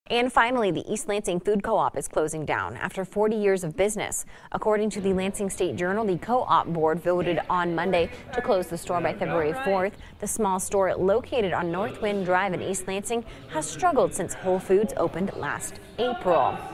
ELFCO closing in East Lansing
The East Lansing Food Co-op is closing down, after 40 years in business. According to the Lansing State Journal, the co-op board voted on Monday to close the store by February 4th. The small store, located on Northwind Drive in East Lansing, has struggled since Whole Foods opened last April.